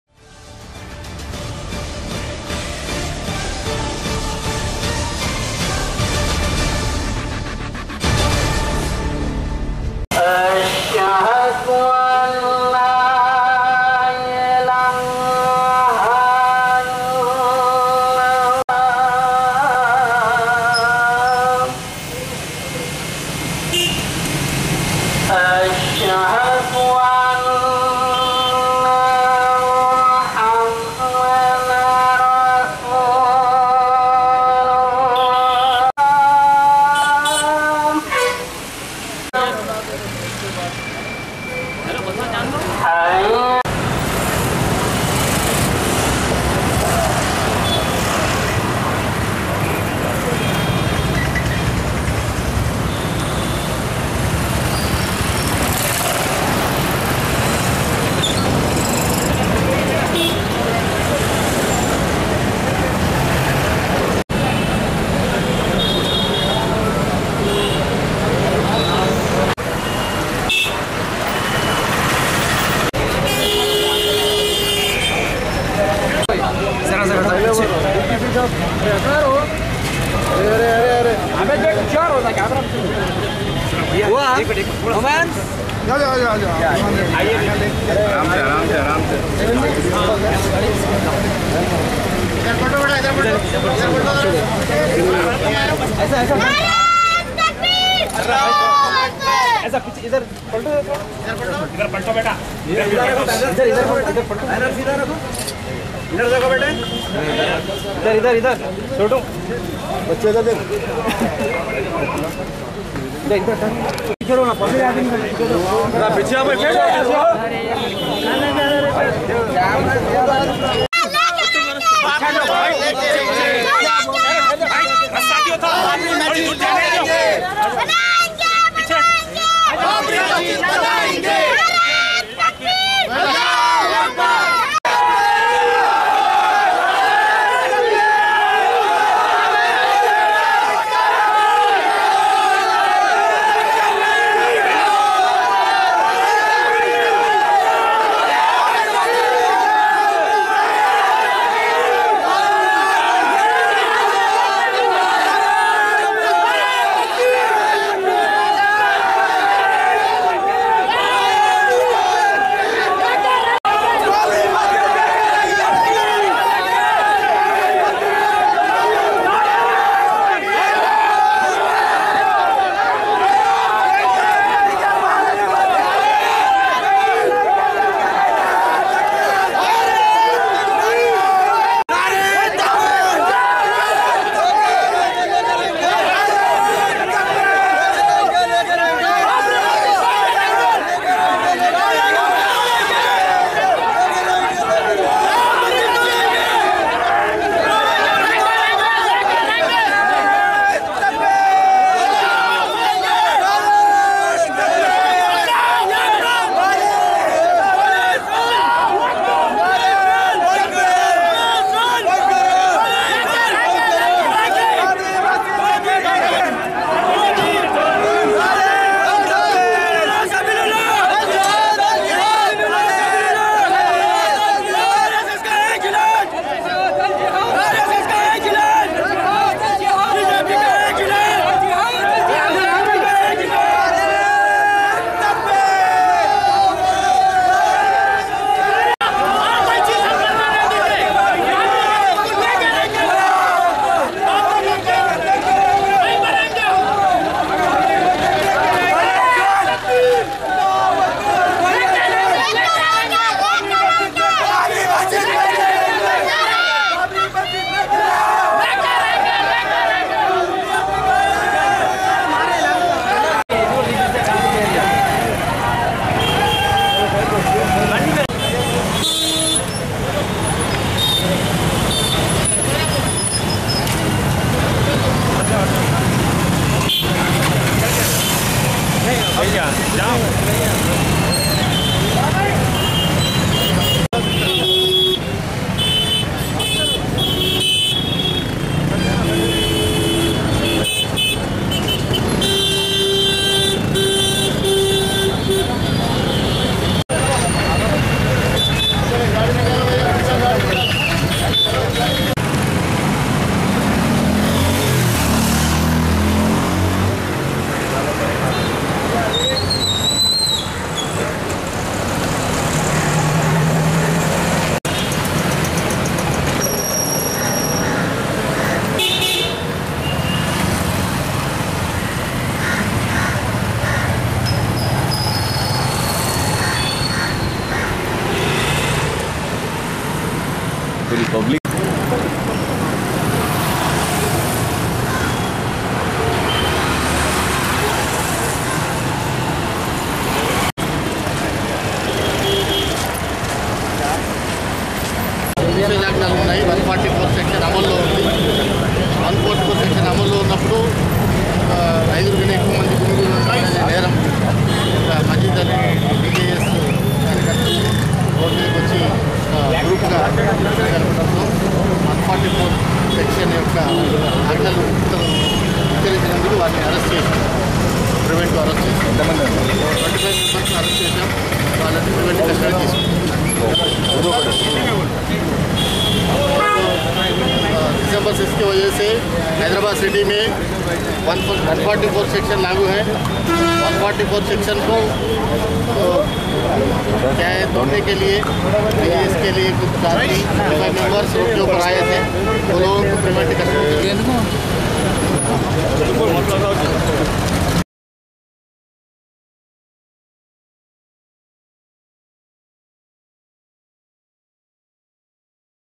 Protest against demolition of BABRI MASJID by DJS-CHOCE NEWS
Protest against Babri Masjid Demolition by Darsgah Jihad o Shahadat (DJS) at Darulshifa, Hyderabad.
Members of DJS were arrested by police.
Report by Lateef Kamran Ansari-Choice News.